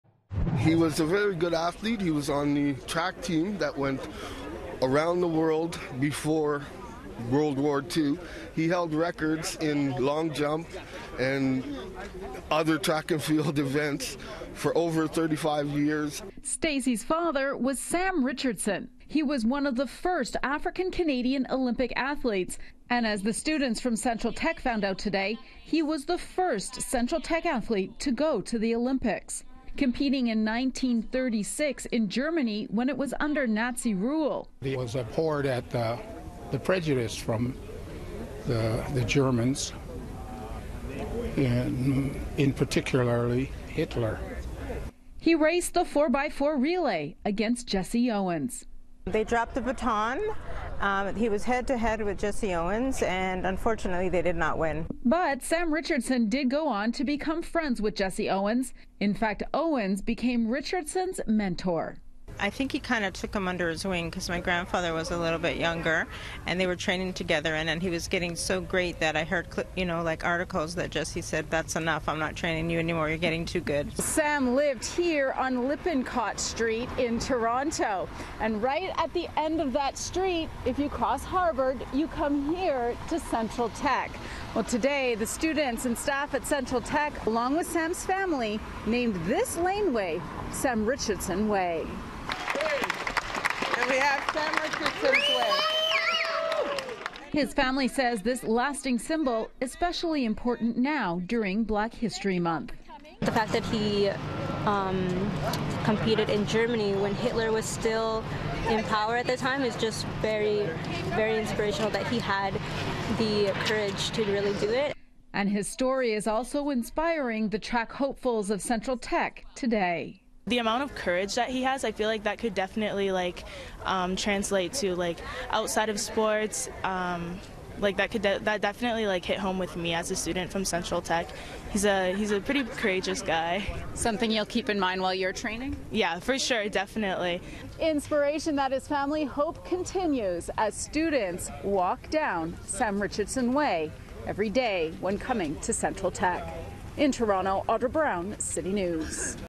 Pathway named after Black Olympian Sam Richardson
A pathway in front of Central Technical School has been named after one of its former athletes, who competed at the 1936 Olympics in Nazi Germany